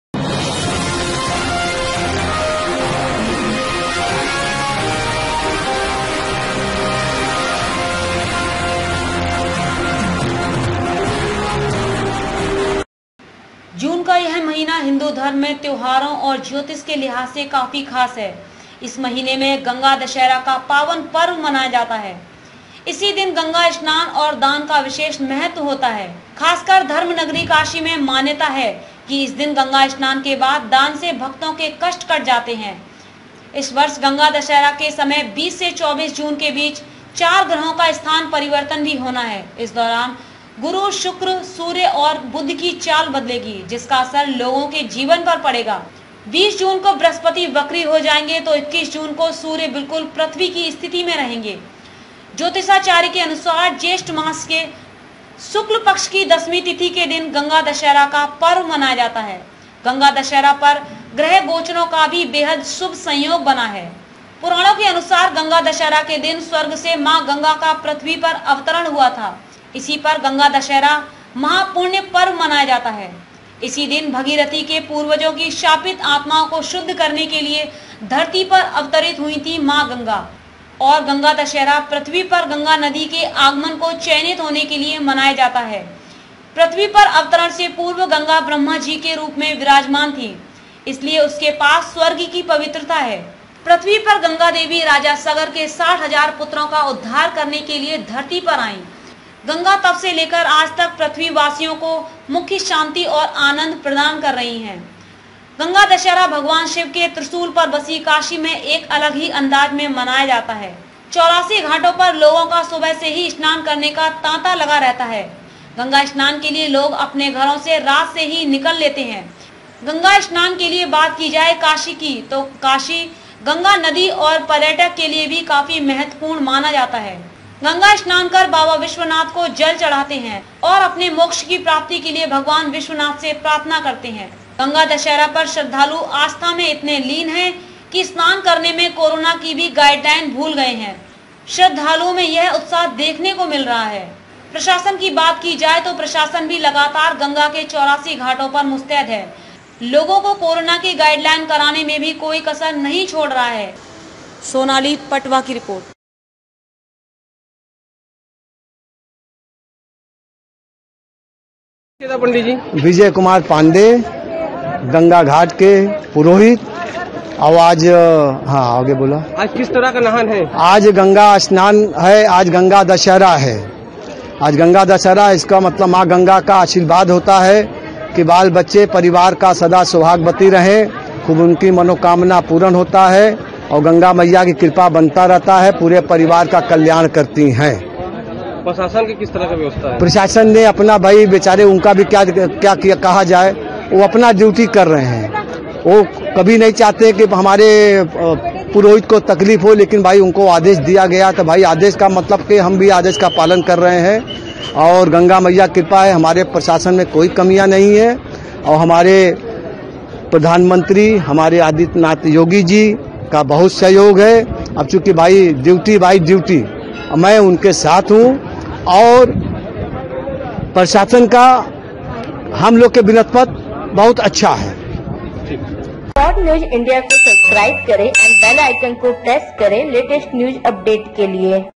Varanasi:- आज है गंगा दशहरा,आज के दिन काशी में स्नान दान करने से मिलता है यह लाभ।।
जून का यह महीना हिंदू धर्म में त्योहारों और ज्योतिष के लिहाज से काफी खास है इसी महीने में गंगा दशहरा का पावन पर्व मनाया जाता है इसी दिन  गंगा स्नान और दान का विशेष महत्व होता है खासकर धर्म नगरी काशी में मान्यता है कि इस दिन गंगा स्नान के बाद दान से भक्तों के कष्ट कट जाते हैं इस वर्ष गंगा दशहरा के समय 20 से 24 जून के बीच 4 ग्रहो का स्थान परिवर्तन भी होना है इस दौरान गुरु, शुक्र ,सूर्य और बुध की चाल बदलेगी जिसका असर लोगों के जीवन पर पड़ेगा 20 जून को बृहस्पति वक्री हो जाएंगे तो 21 जून को सूर्य बिल्कुल पृथ्वी की स्थिति में रहेंगे ज्योतिषाचार्य के अनुसार ज्येष्ठ मास के शुक्ल पक्ष की दशमी तिथि के दिन गंगा दशहरा  का पर्व मनाया जाता है गंगा दशहरा पर ग्रह गोचरो का भी बेहद शुभ संयोग बना है